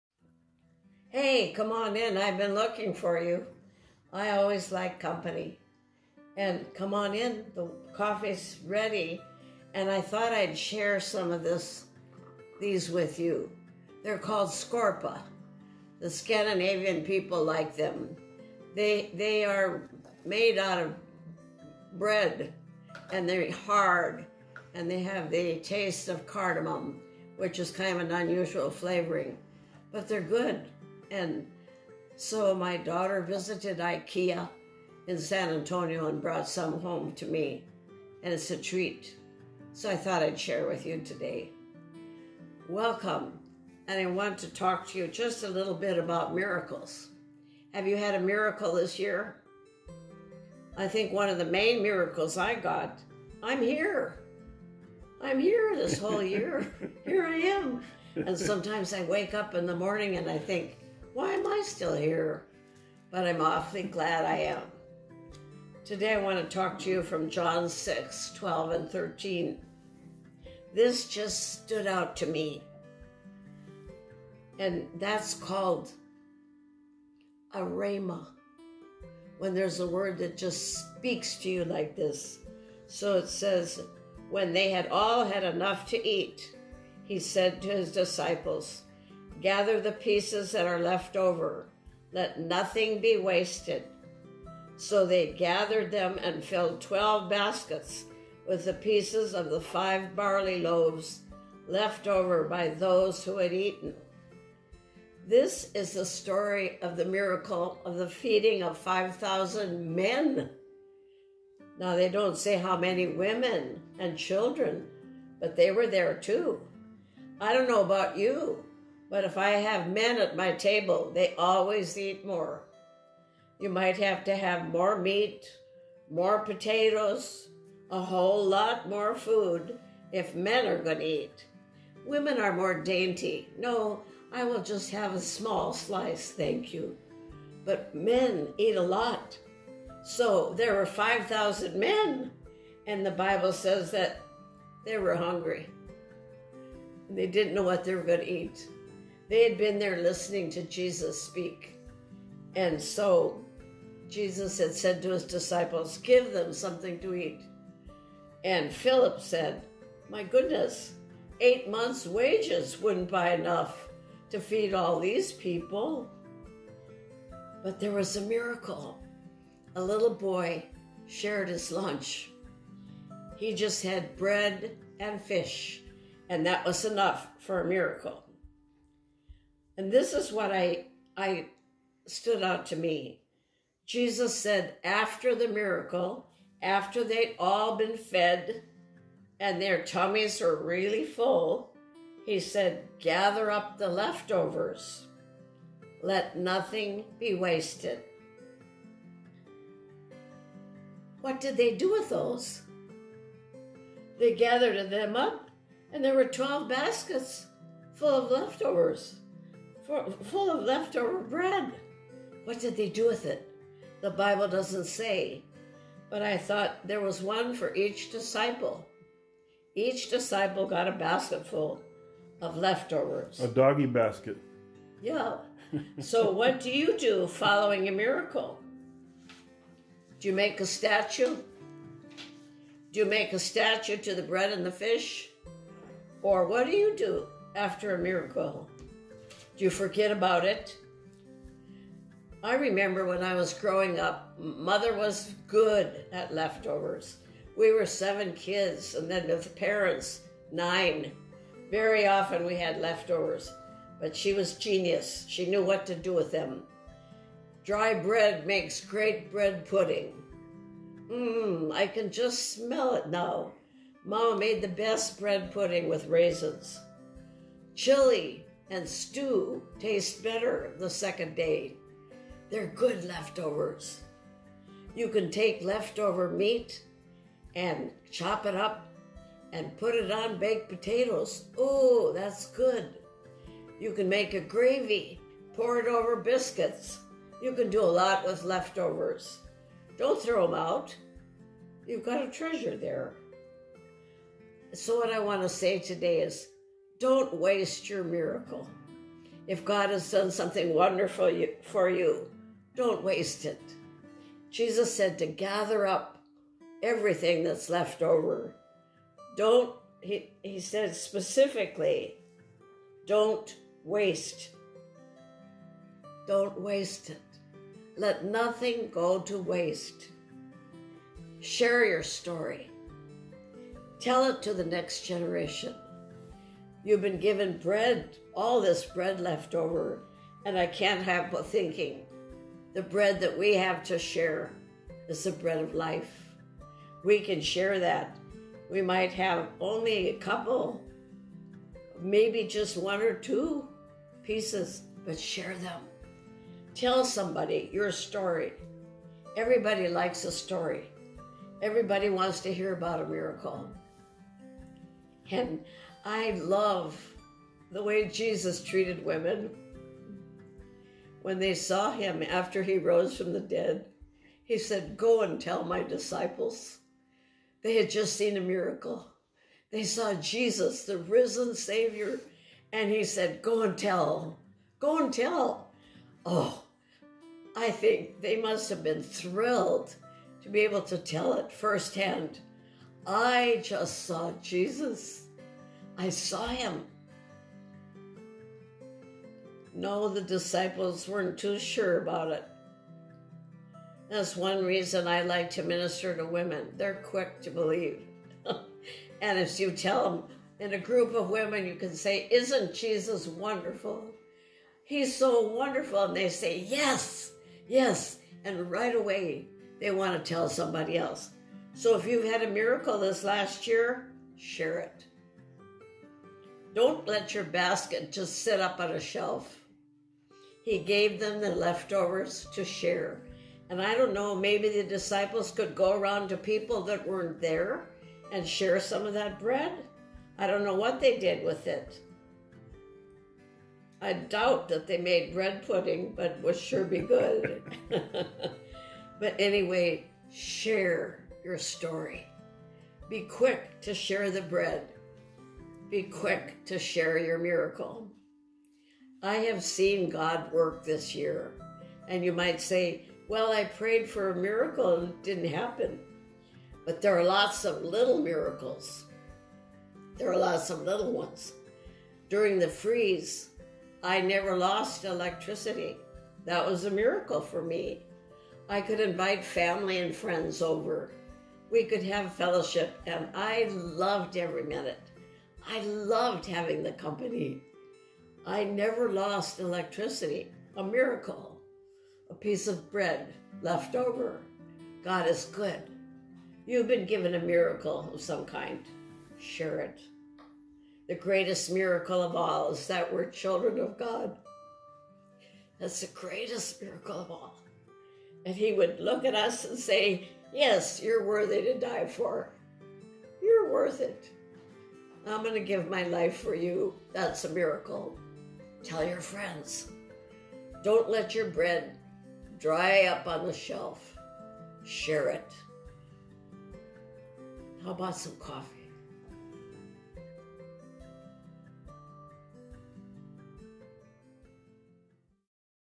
Sharing Our Miracle Leftovers
Clara Ost encourages us to not forget to enjoy and share our “miracle leftovers”, using as an example the 12 baskets of miracle bread left over from Jesus’ feeding of His 5,000+ followers.

When they had all had enough to eat, he said to his disciples, “Gather the pieces that are left over. Let nothing be wasted.”  So they gathered them and filled twelve baskets with the pieces of the five barley loaves left over by those who had eaten. – John 6:12-13

Swedish Skorpor Toast

Coffee with Clara - Episode #40